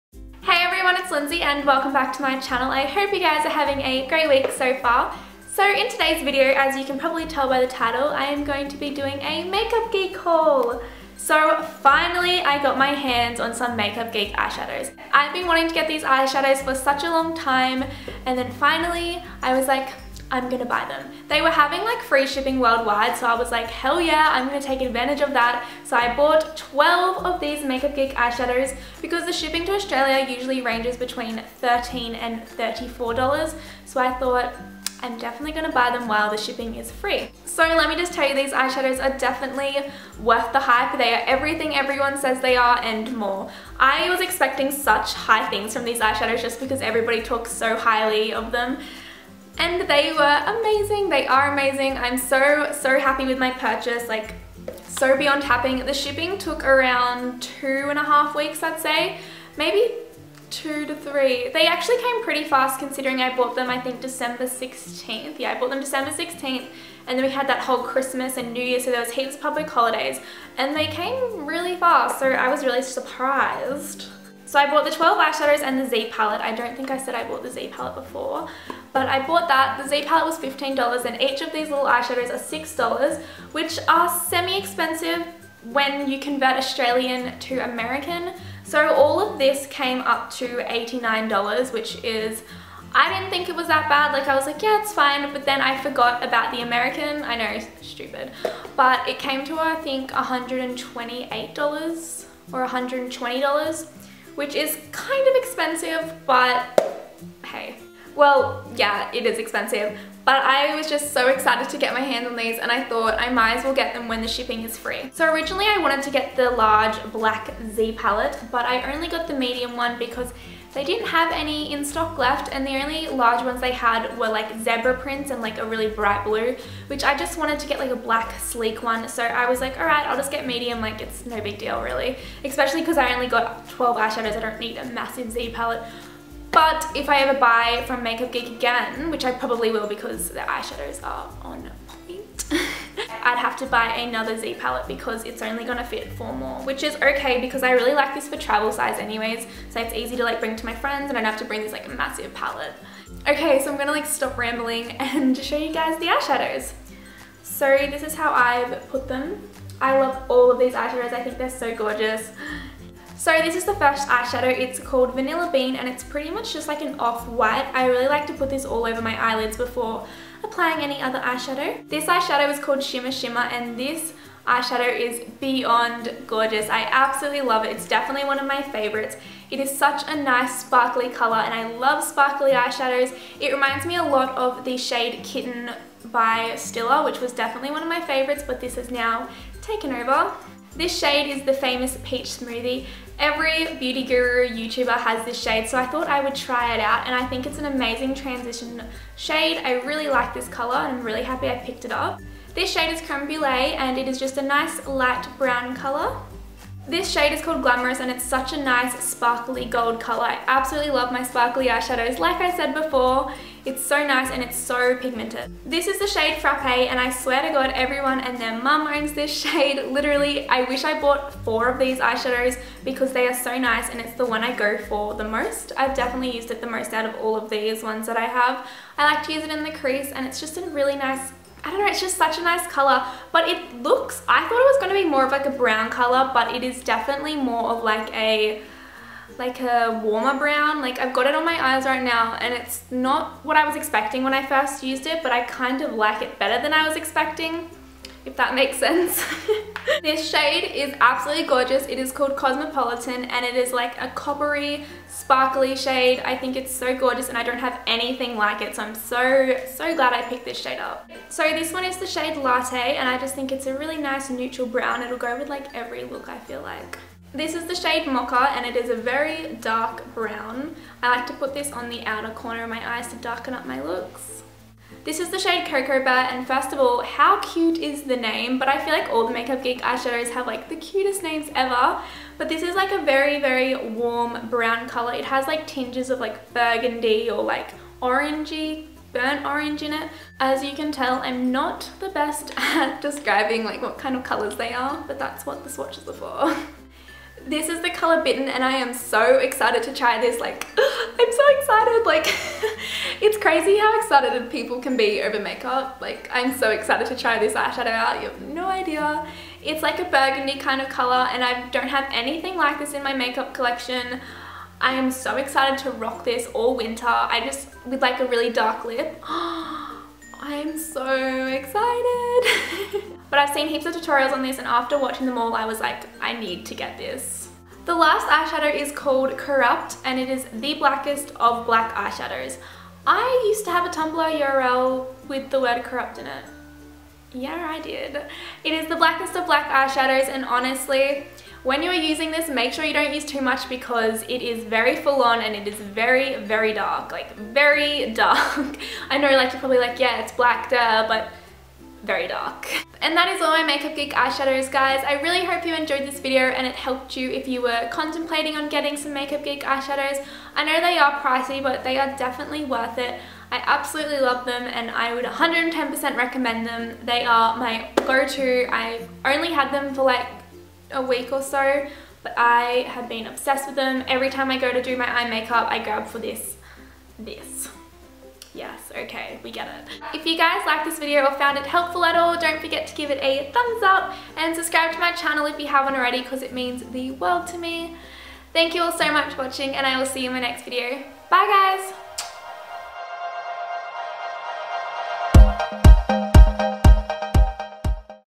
MAKEUP GEEK HAUL + SWATCHES
OPEN FOR MORE INFO & WATCH IN HD PLEASE

Hey babes! Thank you all so much for watching this video, I hope you guys enjoyed this little vlog where amy and i went on a adventure up to springbrook to see the waterfalls and have a lil picnic!! Also if you did let me know by giving me a thumbs up heheh. Hope you all had a wonderful week and ill see you in my next vid xx

PREVIOUS VIDEOS